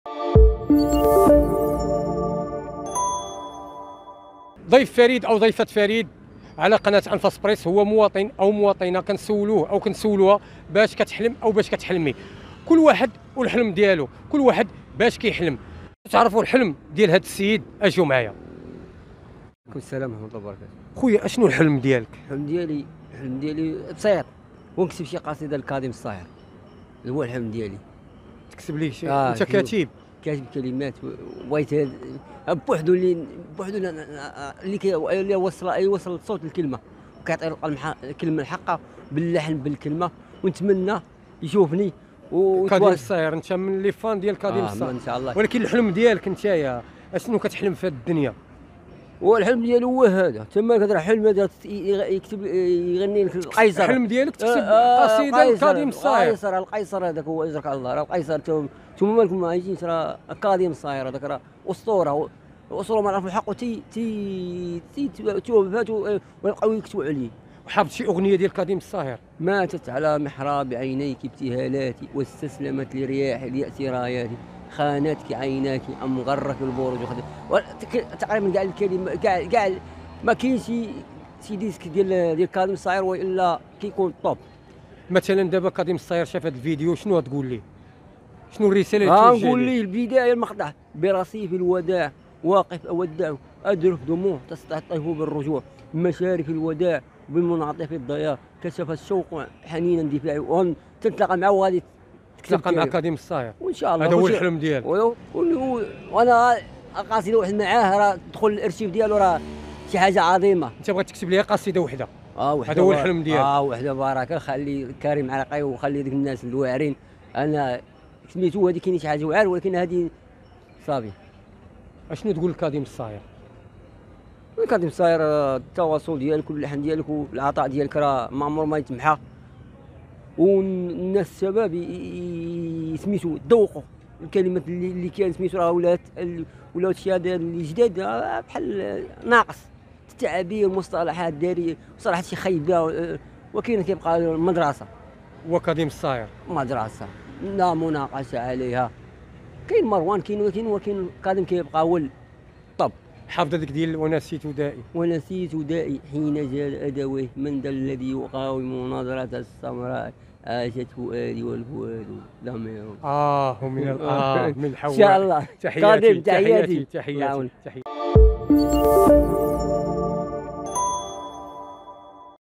أشنو حلمك..مغربي يحلم بكتابة كلمات لأغنية لكاظم الساهر
في ميكرو فريد عبر مواطن مغربي عن عشقه للقيصر الفنان العراقي كاظم الساهر ، وأبدى المستجوب اعجابه الكبير بهذا الفنان الذي أبدع في الأغنية العربية لدرجة أن أقصى ما  يتمناه هو كتابة كلمات كي يغنيها كاظم الساهر .